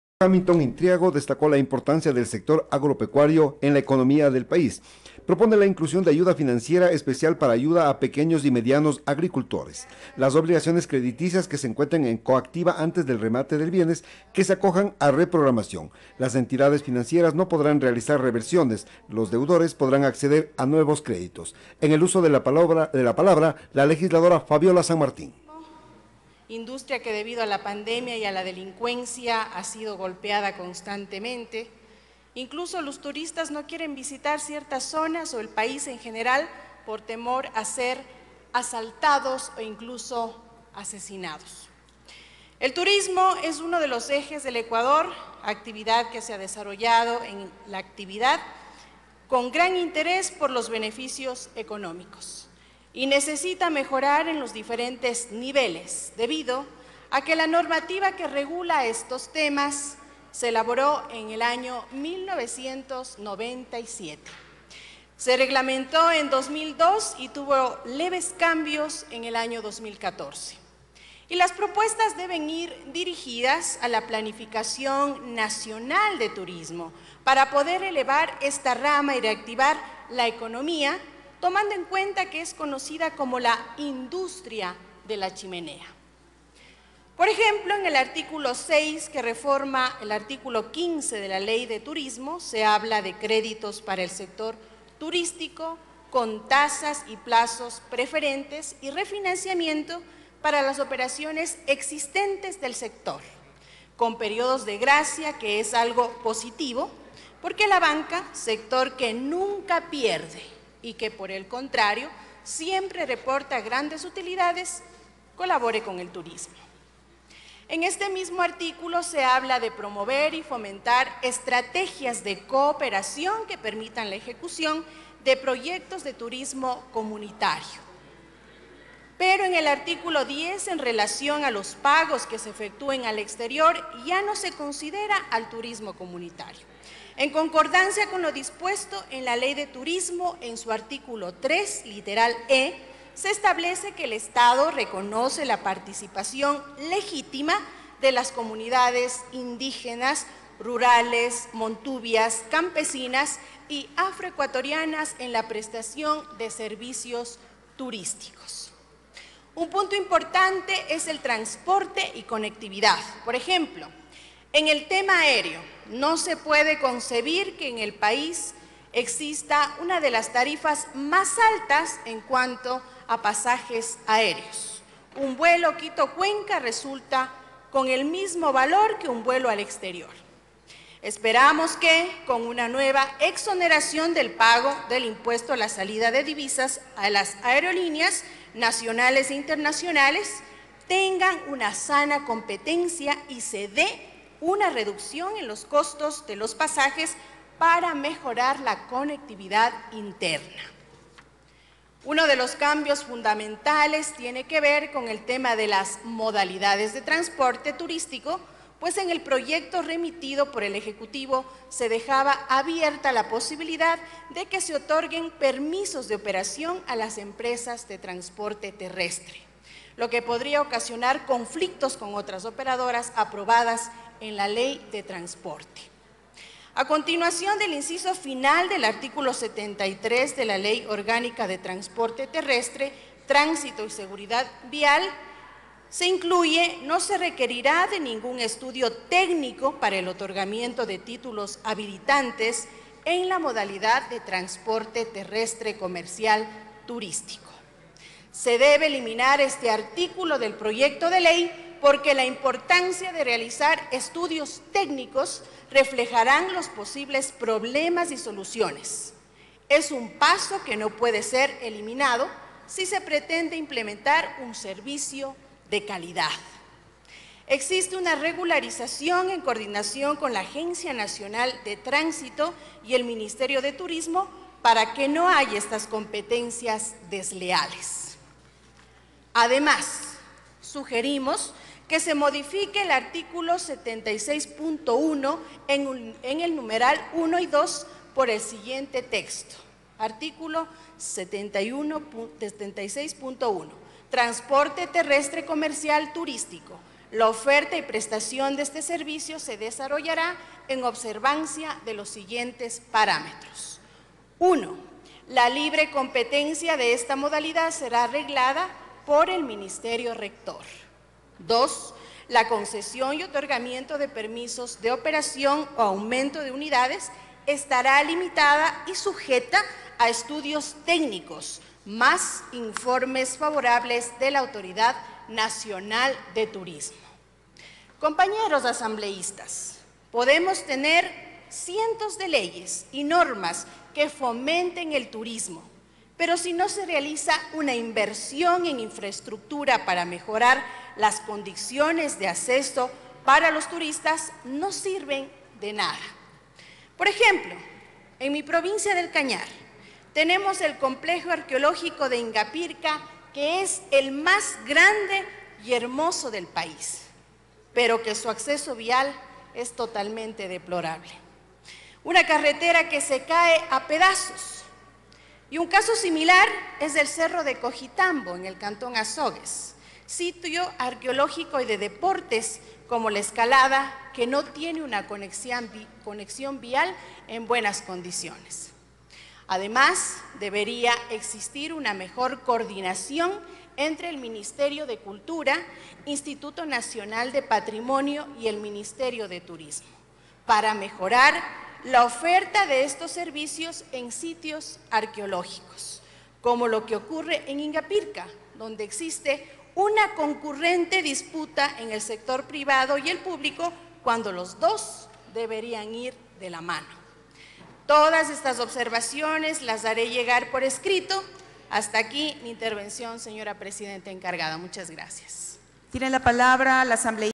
Asambleísta Fabiola Sanmartín -Sesión 914
14 de marzo del 2024
Primer Debate del Proyecto de Ley Orgánica para el Fortalecimiento de la Actividades Turísticas y Fomento del Empleo, calificado como Urgente en Materia Económica.